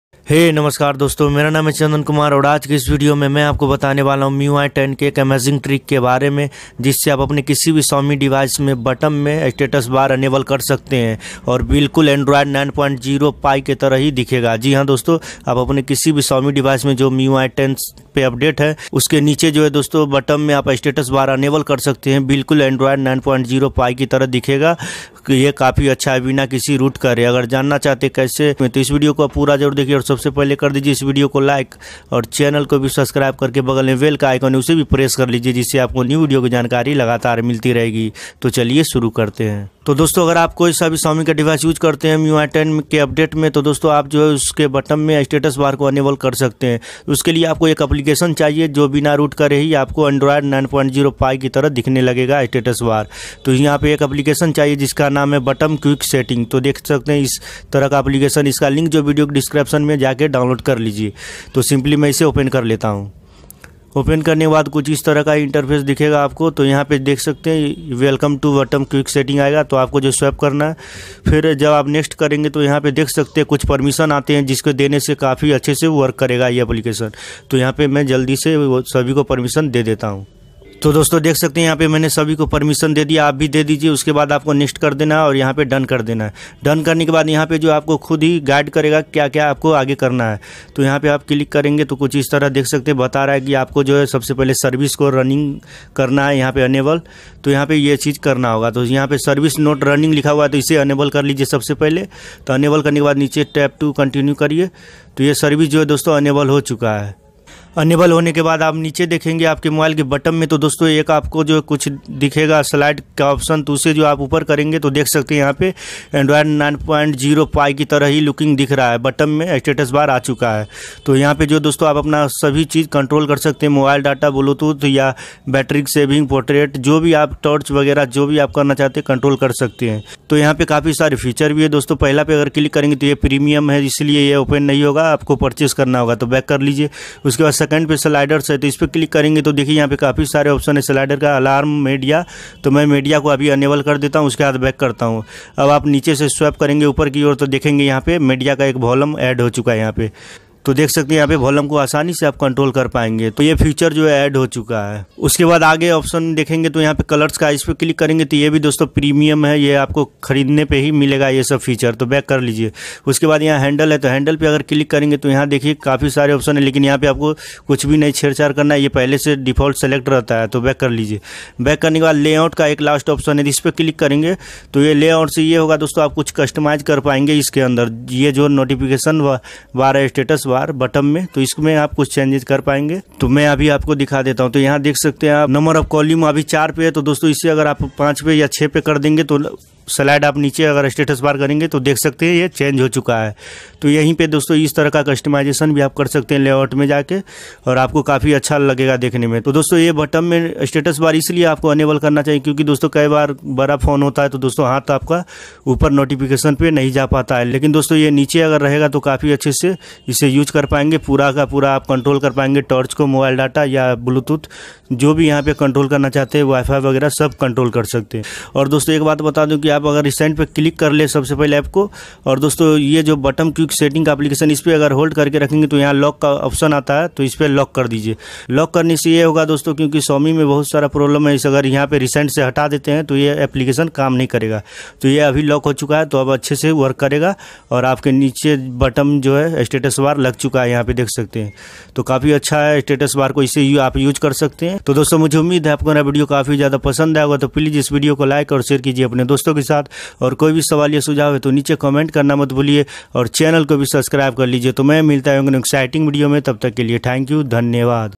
MIUI 10 Buttom Status Bar Enable in Any Xiaomi Phones | MIUI 10 New Update
Namaskaar Dosto, ish video mein maine aapko bataya hai MIUI 10 ke new trick ke baare me jisse aap apne kisi xiaomi phones me buttom notification panel enable kar sakte hai aur android 9.0 pie jaisa look de sakte hai, bina root kare MIUI 10 new update 2019. Mujhe ummid hai aapko yeh video pasand aayegi.

About : Technical Power is a YouTube Channel,
Where you will Find Science & Technological Videos in Hindi, New Video is Posted Everyday :)